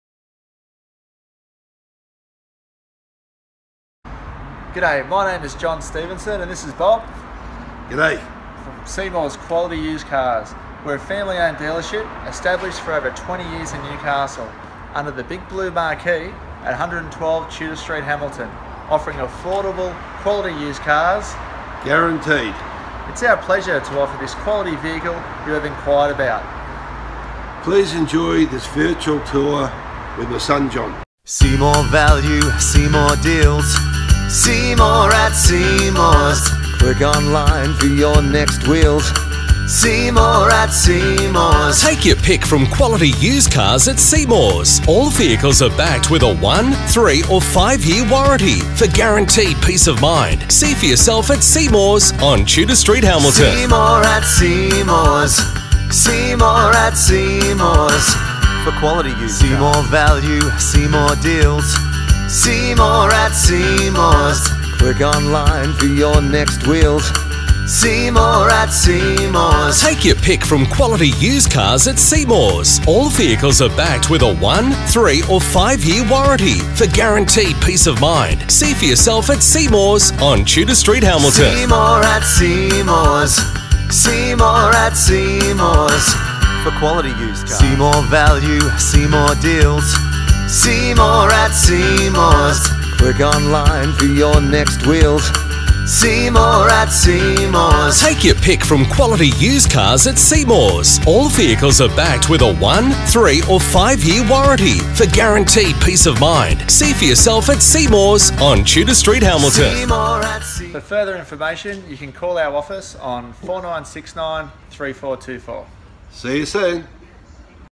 2018 FORD MONDEO AMBIENTE WAGON
11/2018 Ford Mondeo Ambiente Wagon

TOP DOLLAR PAID FOR TRADE IN VEHICLES !!

12 month Australia wide UNLIMITED KM WARRANTY included! Seemore@Seymours.

- 11/2018 Ford Mondeo Ambiente Wagon
- Apple CarPlay
- Amazing Economy Turbo Diesel 5.7ltr/100kms
- 6 Speed Automatic Transmission
- HUGE JANUARY 2024 REGO
- Bluetooth Connectivity with Dual USB ports
- Just serviced with Current Roadworthy Certificate
- Clear PPSR Certificate
- Hill Start Assist
- Cruise Control
- Tinted Windows
- Central Locking with 2 Keys
- Ice Climate Control
- Rear Parking Sensors & Reverse Camera
- Towbar & Roof Racks

Also Featuring -
Dual Front Airbag Package
Airbag Knee Driver
Anti-lock Braking
Auto Climate Control with Dual Temp Zones
Apple Car Play
Adjustable Front Seats - Manual
Alarm System/Remote Anti Theft
Adjustable Steering Wheel - Tilt & Telescopic
Ambient Temperature Display
Android Auto
AUX/USB Input Socket
17 Inch Alloy Wheels
Brake Assist
Body Coloured Exterior Door Handles
Body Coloured Exterior Mirrors
Cruise Control
Cup Holders - Front Seats
Centre Console Box - Multi-purpose
Central Locking Remote Control
Compatible Apps
Child Proof Rear Door Locks
Cloth Trim
Digital Clock
Electronic Brake Force Distribution
Exterior Mirrors - Heated
Electronic Stability Program
Flip/Folding key
Fog Lights - Front
Floor Mats - Front & Rear
Headrests - Adjustable on All Seats
Head Airbags
Headlights - Coming/Leaving Home Function
Hill Holder
Headlights On Warning
High Mounted Rear Stop Light
Headlight Washers System
Illuminated - Entry/Exit with Delayed Fade
Engine Immobiliser
Luggage/Cargo Area Light(s)
Low Fuel Warning
Leather Gear Knob
Leather Steering Wheel
Multi Function Control Screen
Multi Function Steering Wheel
Mobile Phone Connectivity
Map/Reading Lights - Front
Parking Distance Control Rear
Protective Glazing
Projector Headlights
Power Mirrors With Indicators
Power Steering
Power Windows
Radio CD with 6 Speakers
Rear Lights - LED
Rear View Mirror Day/Night
Rear Wiper/Washer
Satellite Navigation
Seatbelts - Height Adjustable Front Seats
Seatbelts - Pre-tensioners Front Seats
Seatbelts - Reminder for All Seats
Side Front Air Bags
Split Fold Rear Seat
Sunglass Holder
Solid Paint
Seatback Pockets - Front Seats
Sunvisors with Vanity Mirrors & Illumination
Spare Wheel - Space Saver/Temporary
SYNC3 Communications System
Trip Computer
Traction Control System
Trailer Stability Control
Voice Recognition System

1-5 yr warranty available for guaranteed peace of mind. Seymours on Tudor,112 Tudor St Hamilton